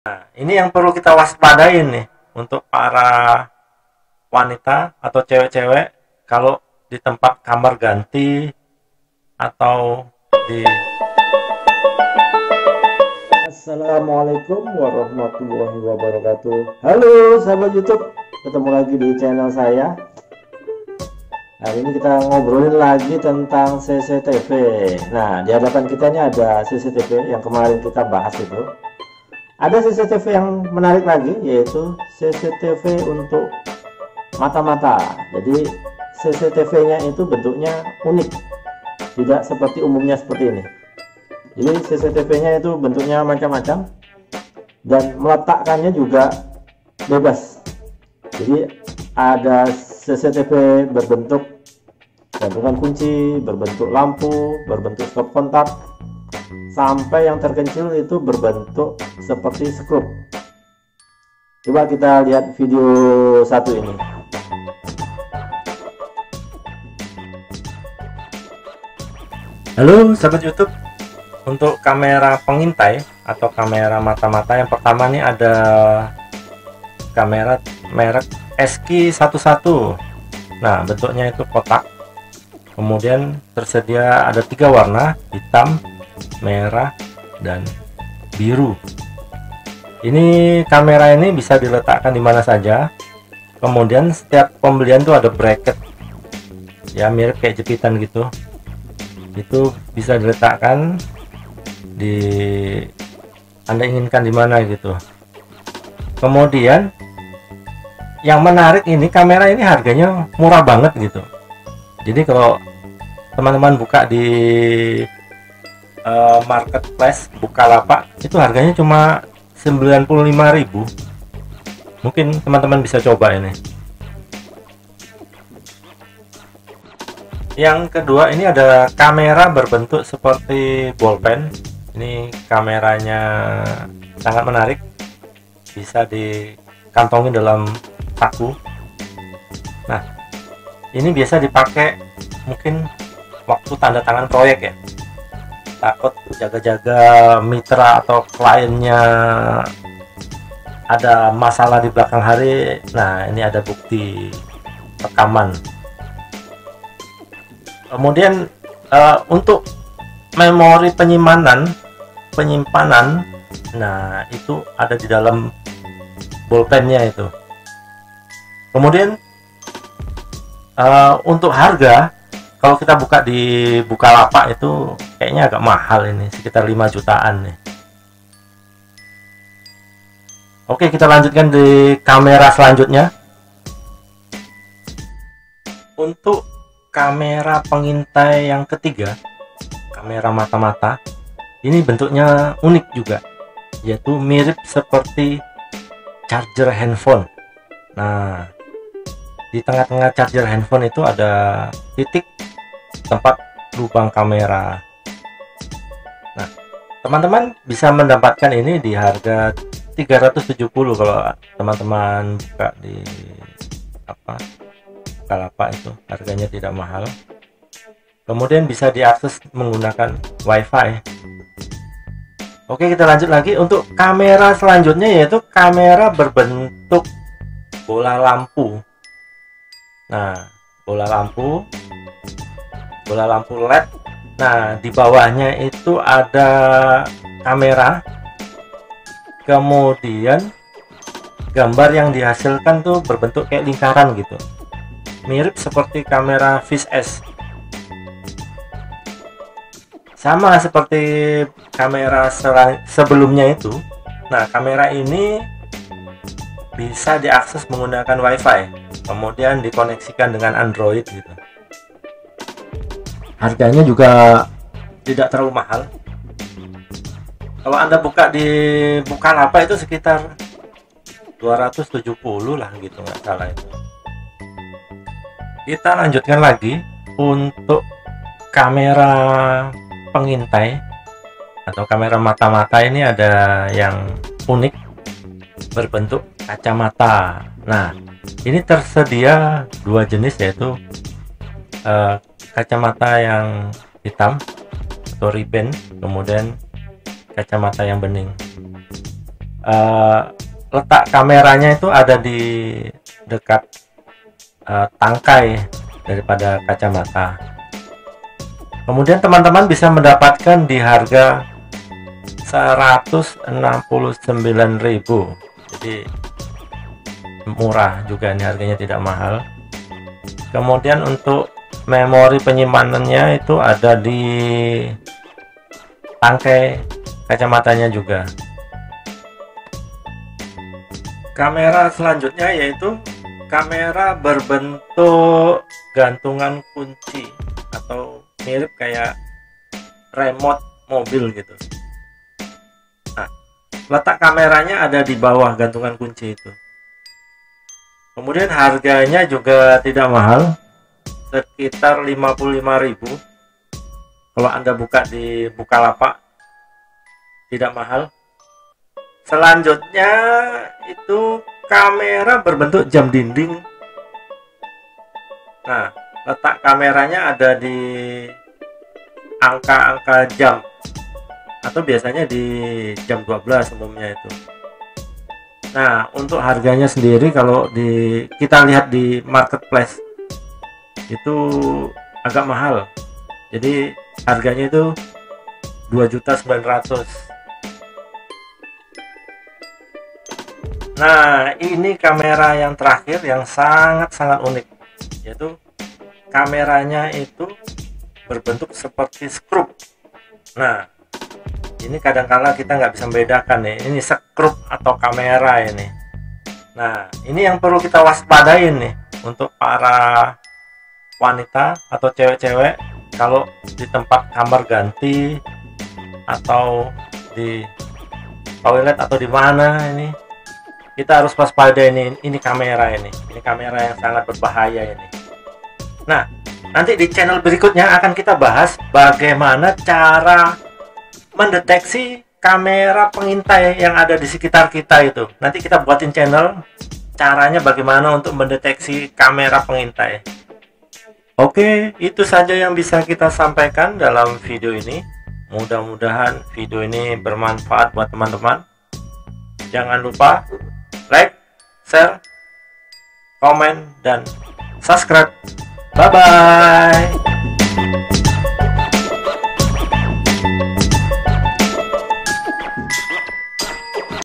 Surveillance camera for spy
Not only action cameras, cameras that are also small in design are spy cameras or surveillance cameras. This camera is usually used to obtain information secretly so it is not surprising if the shape is very unique